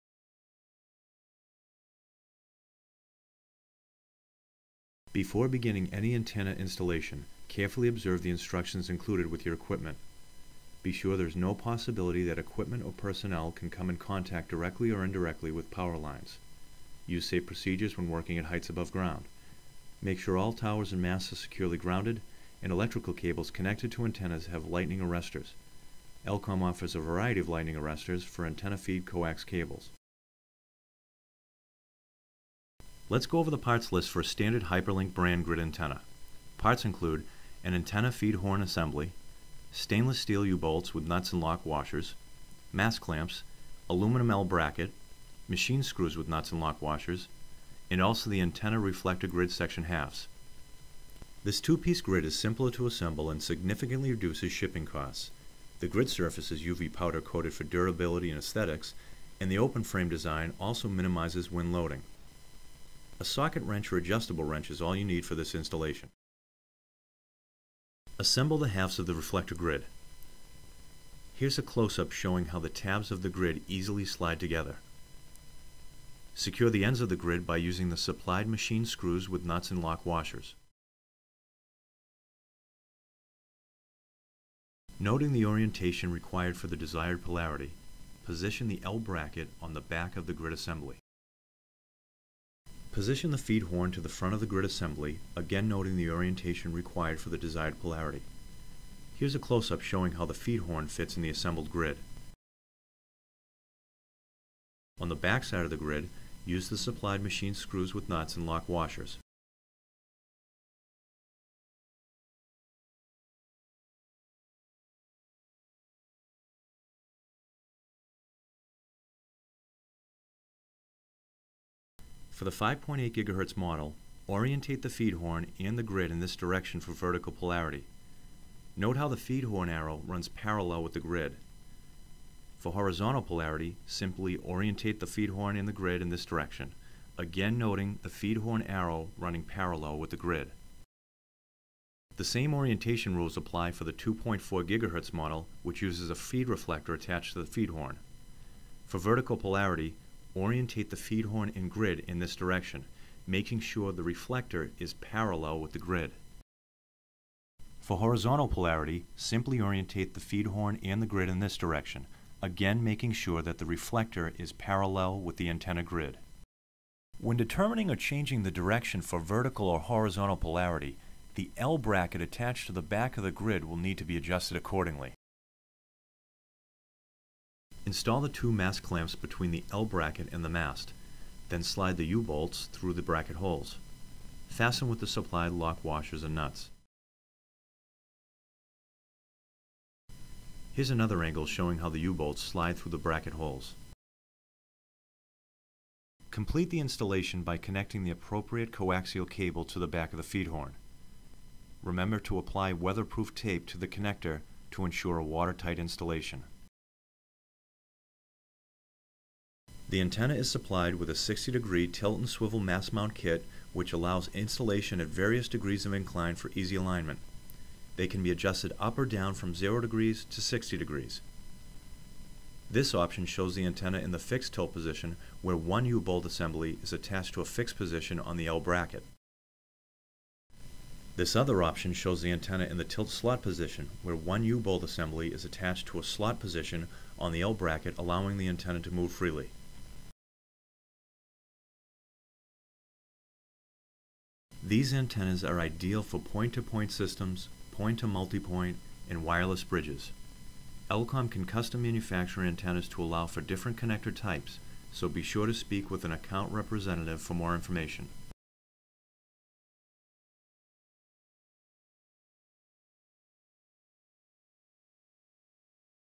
How To Assemble and Install A HyperLink Grid Antenna
L-com Global Connectivity Explains How To Assemble and Install A HyperLink Grid Antenna

Important Safety Precautions

Before beginning any antenna installation, carefully observe the instructions included with your equipment. Be sure there is no possibility that equipment or personnel can come in contact directly or indirectly with power lines.

- Use safe procedures when working at heights above ground.
- Make sure all towers and masts are securely grounded, and electrical cables connected to antennas have lightning arrestors.

L-com offers a variety of lightning arrestors for antenna feed coaxial cables.

Parts List

Let’s go over the parts list for a standard HyperLink Brand Grid antenna.

Parts include:
- Antenna feedhorn assembly
- Stainless steel U-bolts with nuts and flat and lock washers
- Mast clamps - Aluminum L bracket
- Machine screws with nuts and flat and lock washers
- And also, the antenna reflector grid section halves

The 2-piece grid is simple to assemble and significantly reduces shipping costs. The grid surface is UV powder coated for durability and aesthetics and the open-frame design also minimizes wind loading.

A socket wrench or adjustable wrench is all you need for this installation.

Step 1 Assemble the Grid

Assemble the halves of the reflector grid. Here’s a close up showing how the tabs of the grid easily slide together. Secure the grid by using the supplied machine screws with nuts and flat and lock washers.

Step 2 Position the L Bracket to the Back Noting the orientation required for the desired polarity, position the L bracket to the back of the grid assembly.

Step 3 Position the Feedhorn to the Front Position the feedhorn to the front of the grid assembly, again noting the orientation required for the desired polarity. Here’s a closeup showing how the feedhorn fits in the assembled grid. On the back side of the grid, use the supplied machine screws with nuts and flat and lock washers.

Step 4 Determine the Polarity For the 5.8 GHz model, orientate the feedhorn and the grid in this direction for vertical polarity. Note how the feedhorn arrow runs parallel with the grid. For horizontal polarity, simply orientate the feedhorn and the grid in this direction again noting the feedhorn arrow running parallel with the grid. The same orientation rules apply for the 2.4 GHz model which uses a feed reflector attached to the feedhorn. For vertical polarity, orientate the feedhorn and grid in this direction making sure the reflector is parallel with the grid. For horizontal polarity, simply orientate the feedhorn and the grid in this direction again making sure that the reflector is parallel with the antenna grid.

When determining or changing the direction for vertical or horizontal polarity, the "L" bracket attached to the back of the grid will need to be adjusted accordingly.

Step 5 Install Mast Clamps Install the two mast clamps between the L bracket and the mast. Then slide the U-bolts through the bracket holes. Fasten with the supplied lock washers and nuts. Here’s another angle showing how the U-bolts slide through the bracket holes.

Step 6 Complete the Installation By Connecting the Appropriate Cable Complete the installation by connecting the appropriate coaxial cable to the back of the feedhorn. Remember to apply weatherproof tape to the connector to ensure a watertight installation.

60° Optional Tilt Mounting

This grid antenna is supplied with a 60 degree tilt and swivel mast mount kit which allows installation at various degrees of incline for easy alignment. They can be adjusted up or down from 0° to 60°. This option shows the antenna in the fixed tilt position where one U-bolt assembly is attached to a fixed position on the "L" bracket. This other option shows the antenna in the tilt slot position where one U-bolt assembly is attached to a slot position on the "L" bracket allowing the antenna to move freely.

These Hyperlink antennas are ideal for point-to-point systems, point to multi-point and wireless bridges.

L-com can custom manufacture Wi-Fi antennas to allow for different connector types so be sure to speak with an account representative for more information.

View more L-com wired and wireless connectivity tips and videos